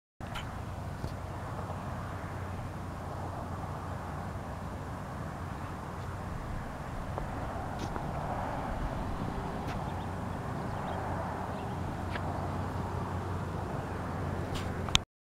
Vromans Nose - Dance Floor
Views from the Dance Floor on Vromans Nose in Schoharie County.